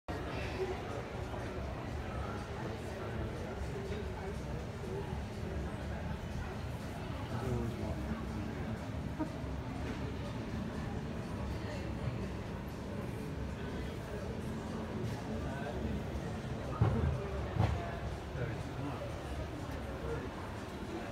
D154 Archipelago Handcrafted Wooden Bird Sculpture Barn Owl
D154 Archipelago Handcrafted Wooden Bird Sculpture Barn Owl is a very popular piece showing this bird of prey with a beautiful finish. Click here to view the Archipelago Bird Collection